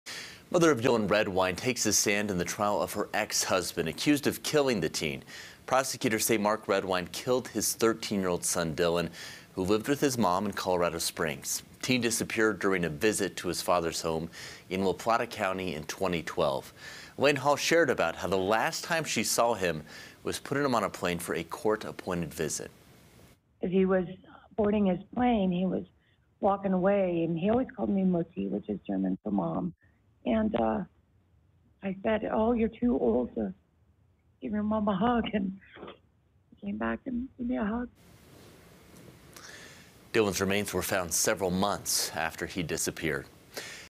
Mother Of Dylan Redwine Took The Stand Today In The Trial Of Her Ex-Husband
Elaine Hall shared with the court the last time she saw her son alive.